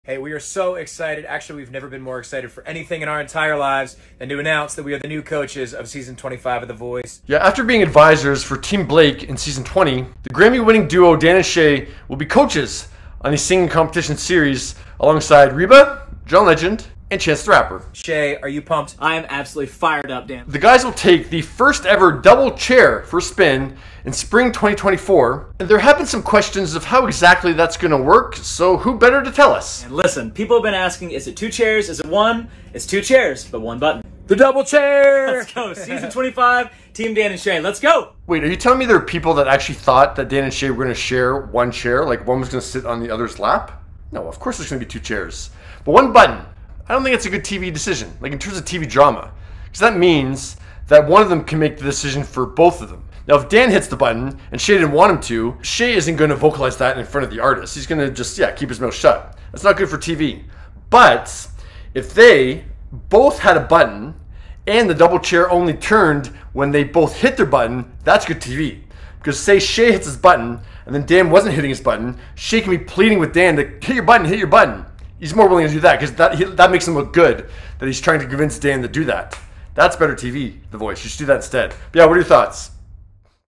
Dan And Shay Explain 'Voice' Double Chair
After Dan and Shay clarify how the "Double Chair" will work on season 25 of "The Voice", Graeme O'Neil explains why he disagrees.